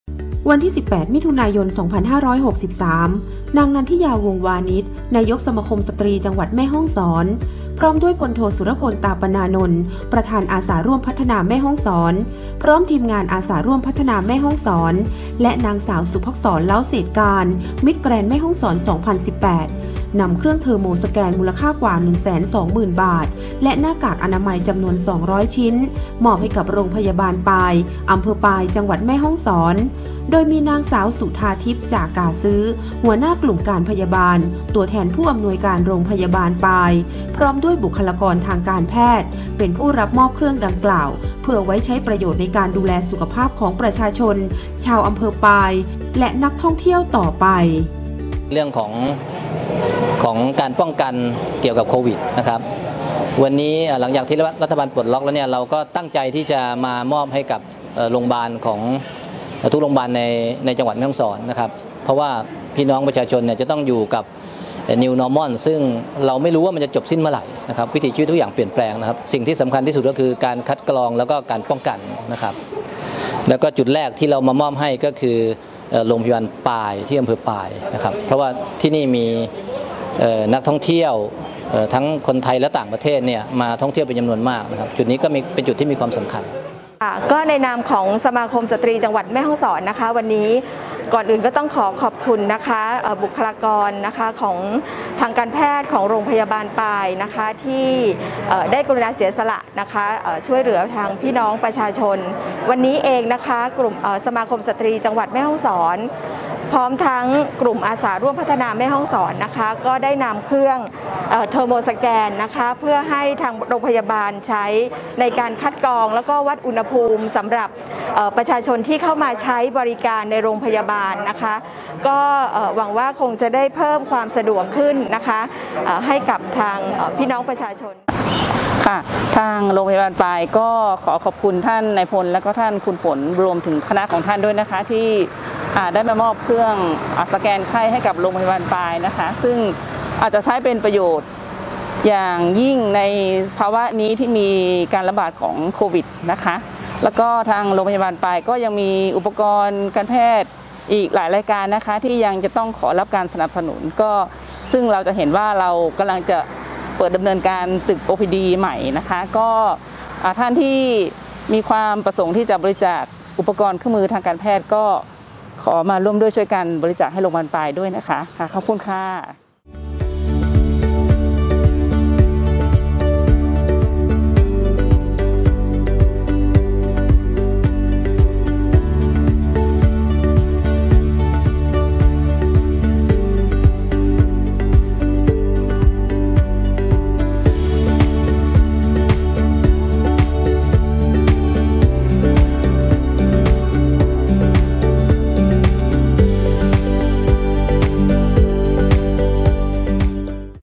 ข่าวMSHNEWSมอบเครื่องสแกน รพ ปาย
ข่าวMHSNEWสมาคมสตรีจังหวัดแม่ฮ่องสอน พร้อมทีมงานอาสาร่วมพัฒนาแม่ฮ่องสอน เดินหน้าพัฒนาบ้านเกิด มอบเครื่อง เทอร์โมสแกน พร้อมหน้ากากอนามัย ให้กับ รพ.ปาย
วันที่ 18 มิถุนายน 2563 นางนันทิยา วงศ์วานิชย์ นายกสมาคมสตรีจังหวัดแม่ฮ่องสอน พร้อมด้วยพลโทสุรพล ตาปนานนท์ ประธานอาสาร่วมพัฒนาแม่ฮ่องสอน พร้อมทีมงานอาสาร่วมพัฒนาแม่ฮ่องสอน และนางสาวศุภักษร เล้าเศษการ มิสแกรนด์แม่ฮ่องสอน 2018 นำเครื่องเทอร์โมสแกน มูลค่า 120,000  บาท  (หนึ่งแสนสองหมื่นบาท ) และ หน้ากากอนามัย 200  ชิ้น ( มีใบรับรอง ) มอบให้กับโรงพยาบาลปาย อ.ปาย จ.แม่ฮ่องสอน โดยมี นางสาวสุทาทิพย์ จ่าก่าซื้อ หัวหน้ากลุ่มการพยาบาล ( ตัวแทน ผู้อำนวยการ รพ.ปาย ) พร้อมด้วยบุคลากรทางการแพทย์ เป็นผู้รับมอบเครื่องดังกล่าวไว้ใช้ประโยชน์ในการดูแลสุขภาพประชาชนชาวอำเภอปาย และนักท่องเที่ยว.
นางนันทิยา วงศ์วานิชย์ นายกสมาคมสตรีจังหวัดแม่ฮ่องสอน กล่าวว่า ด้วยสำนึกรักบ้านเกิด และมีความตั้งใจให้โรงพยาบาลแต่ละโรงพยาบาลในจังหวัดแม่ฮ่องสอน มีเครื่องมือทางการแพทย์ที่ทันสมัย ทัดเทียมกับจังหวัดอื่นๆ เพื่อความสะดวกต่อการปฏิบัติหน้าที่ของบุคคลากรทางการแพทย์ และเพื่อไว้ใช้คัดกรอง และวัดอุณหภูมิ  สำหรับประชาชนและนักท่องเที่ยวที่ต้องไปใช้บริการของโรงพยาบาล
ด้านพลโทสุรพล ตาปนานนท์ ประธานอาสาร่วมพัฒนาแม่ฮ่องสอน ได้กล่าวว่า ประเด็นสำคัญ คือทุกฝ่ายต้องร่วมมือกัน ในเรื่องของการป้องกันโรคไวรัสโคโรน่า 2019 หรือ โควิด 19  ซึ่งในครั้งนี้ สมาคมสตรีจังหวัดแม่ฮ่องสอน และทีมงานอาสาร่วมพัฒนาแม่ฮ่องสอน มีความตั้งใจที่จะมอบเครื่องเทอร์โมสแกน จำนวน 7 เครื่อง ให้กับโรงพยาบาลทั้ง 7 อำเภอของจังหวัดแม่ฮ่องสอน  เพื่อให้เกิดประโยชน์กับพี่น้องประชาชนทั้ง 7 อำเภอ ในการป้องกันโรคโควิด 19  ต่อไป